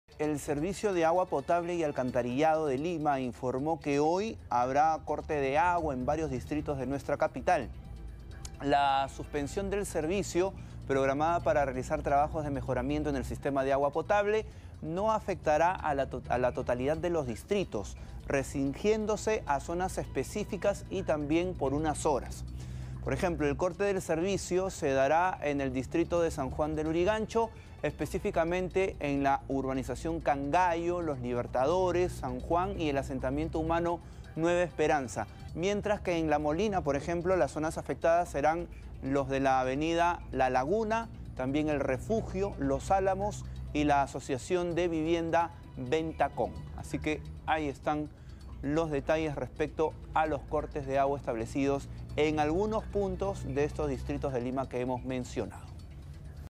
Sedapal anunció corte de agua en algunos distritos de Lima
La interrupción del servicio se efectuará en algunas zonas de San Juan de Lurigancho y La Molina.